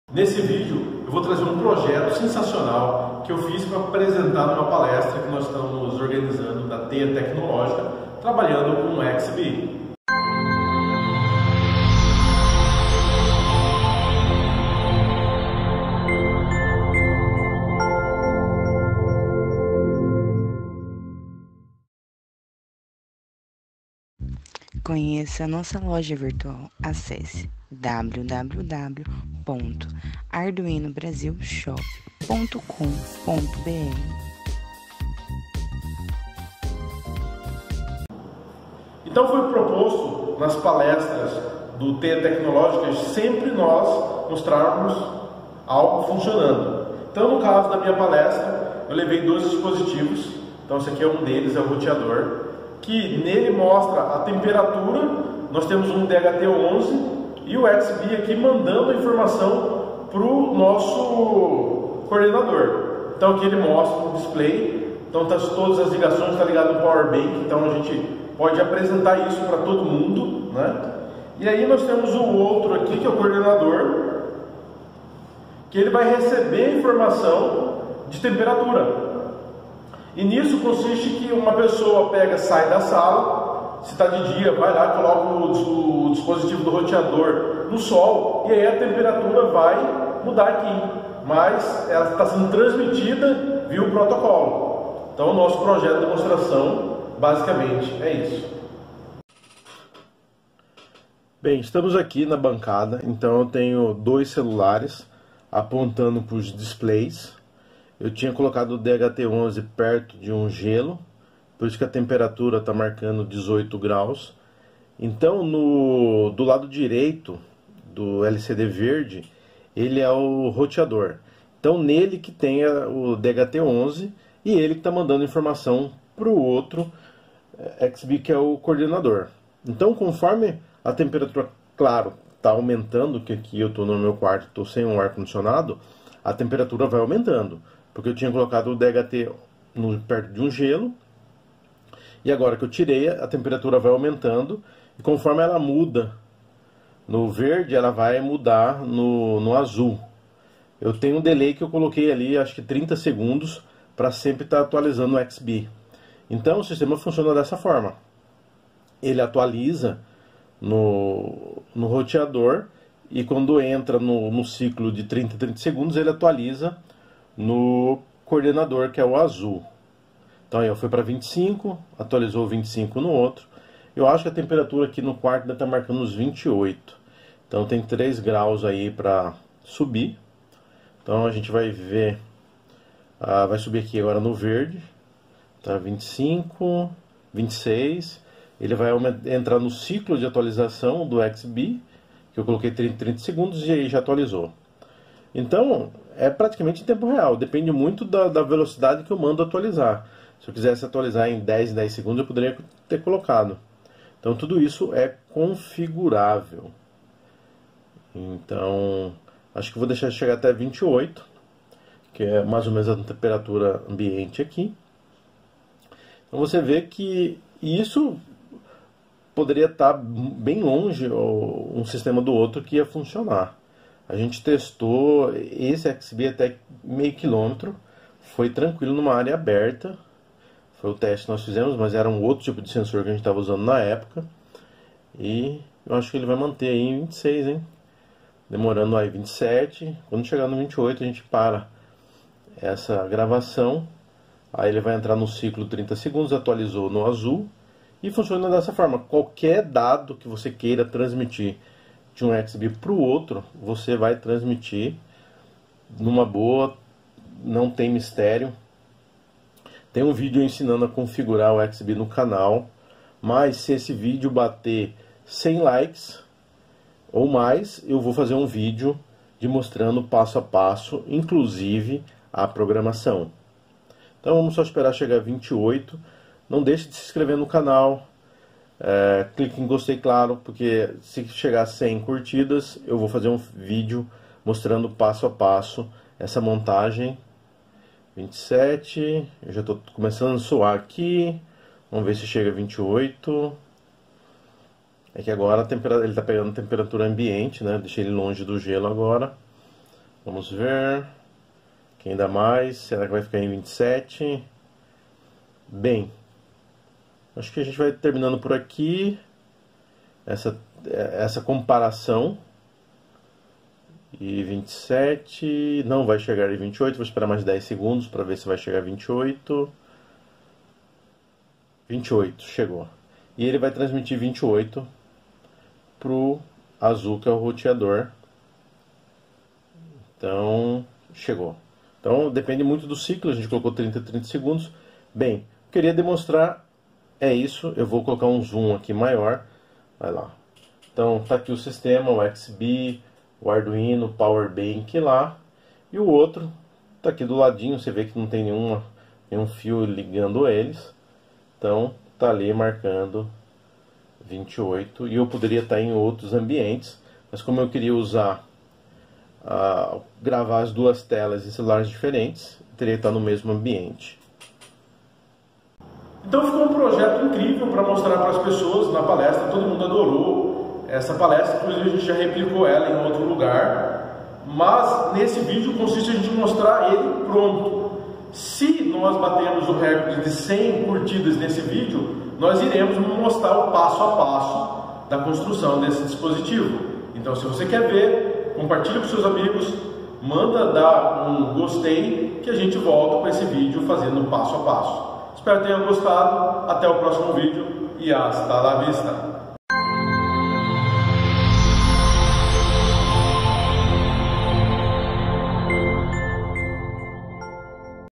TEIA TECNOLÓGICA - PROJETO DE TCC UTILIZANDO XBEE
Projeto do Nascimento, que foi demostrada na Palestra do Teia Tecnológica utilizando Xbee.